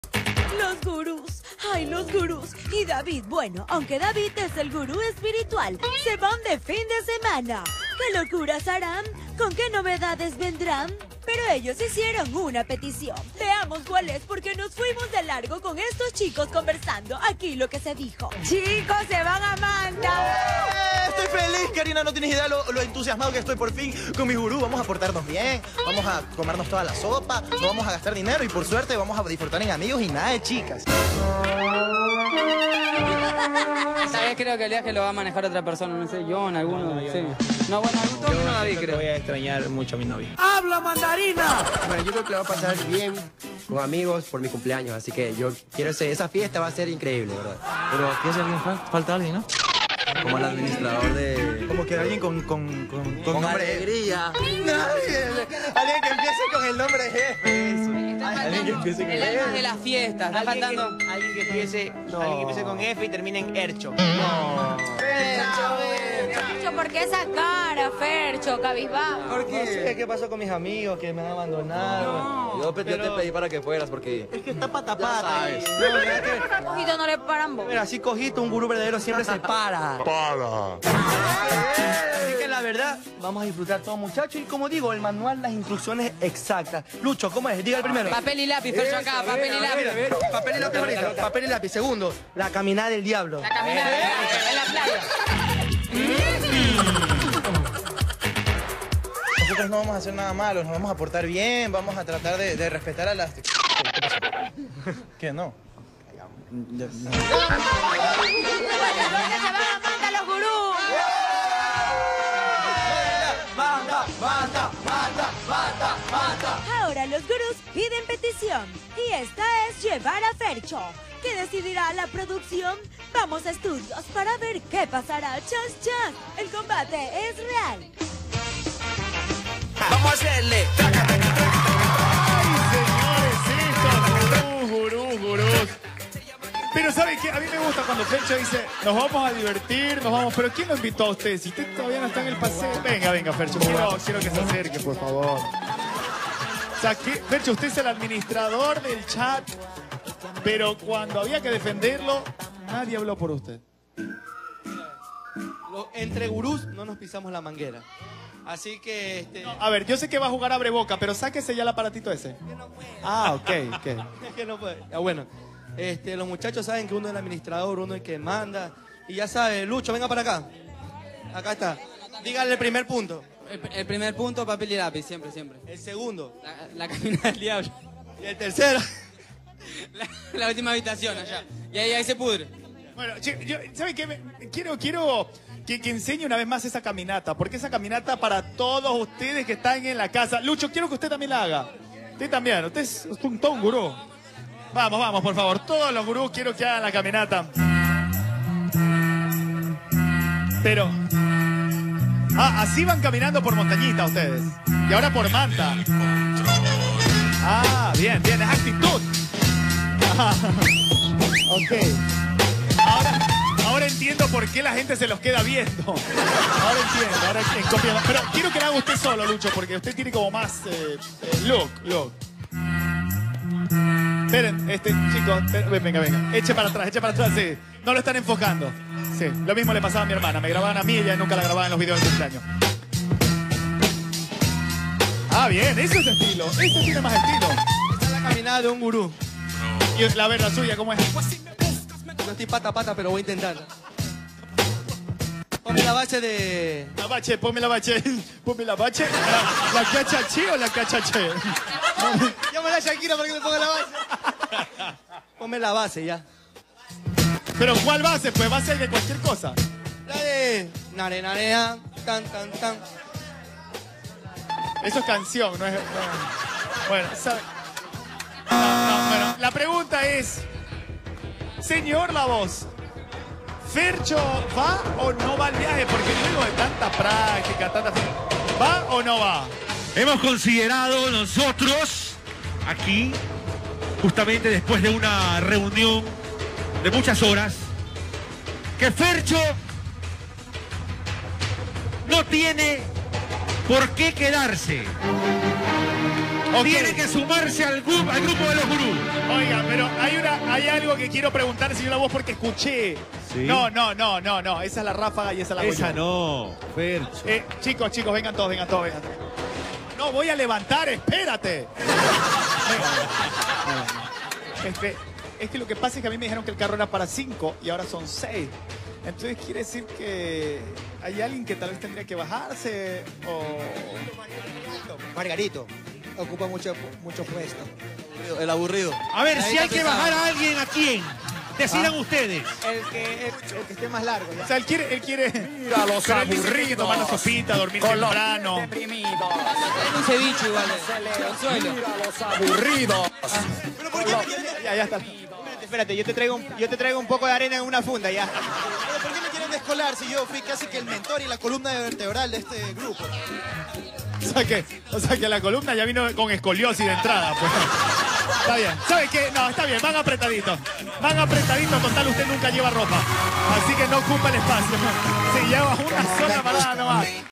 Los gurúes se van a Manta
Los gurúes visitarán Manta, en un nuevo fin de semana llenas de locuras, hablamos con ellos antes, para escuchar la especial petición que nos hicieron.

¿Qué ocurrencias pasarán?